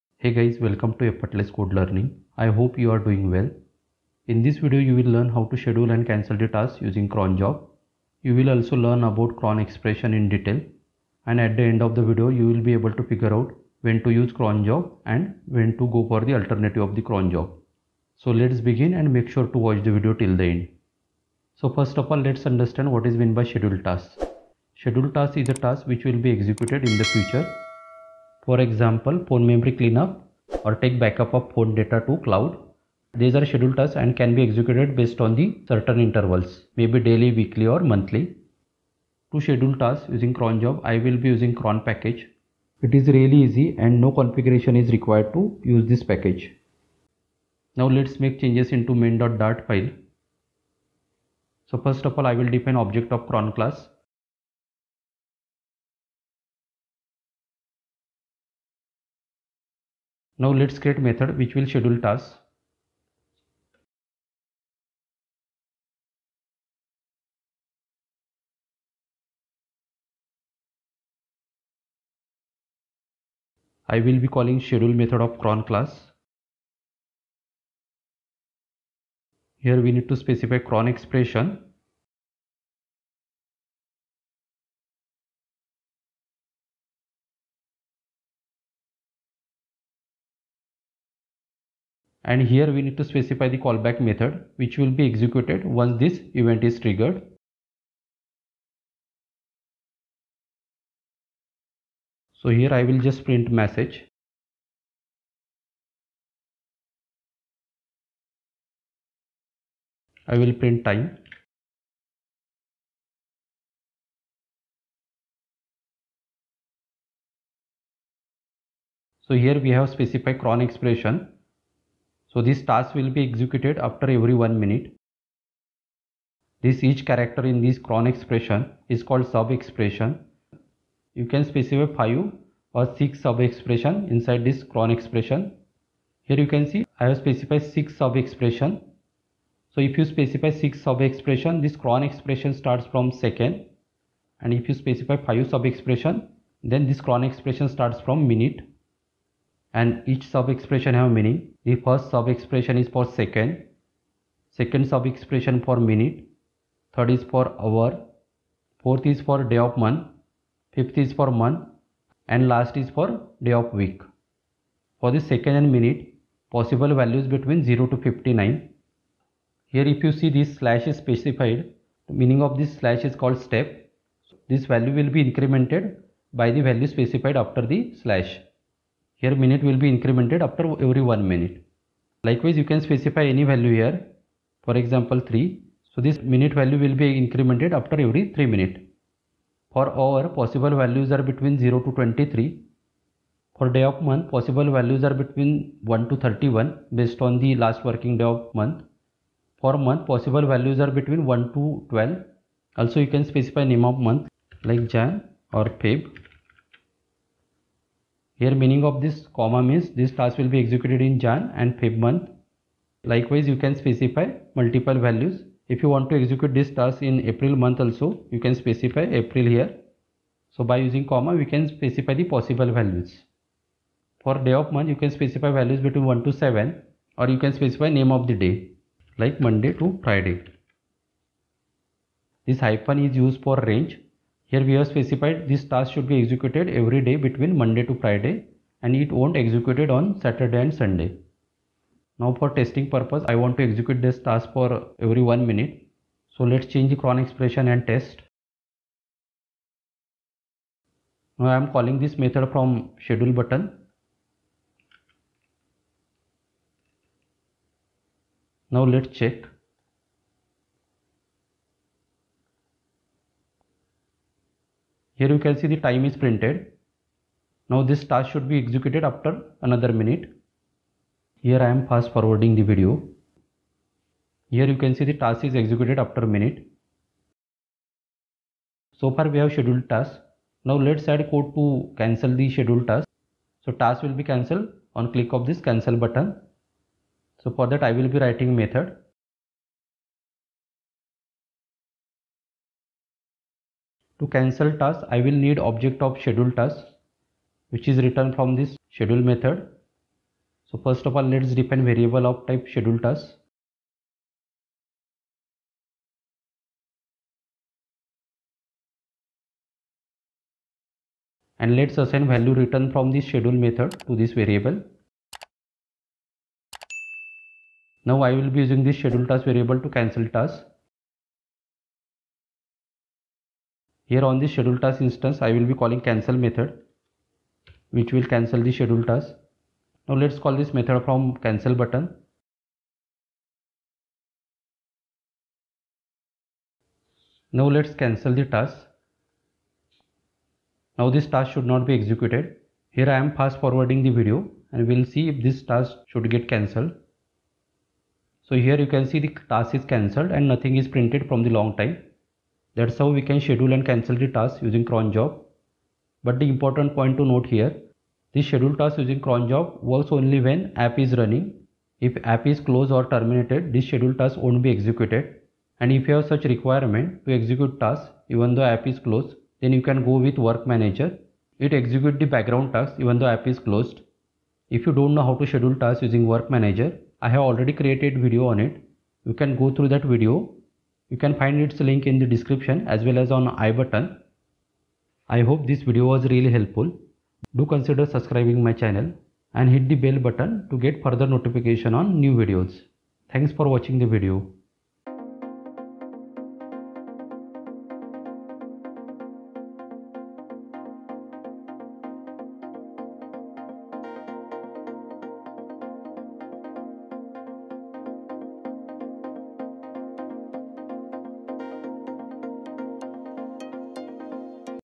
Flutter - Schedule Task Using Cron Job Scheduler [2022]
This video covers how to schedule & cancel task using cron job. It also covers details about cron  expression. By watching this video, we will understand the suitable use case for cron job.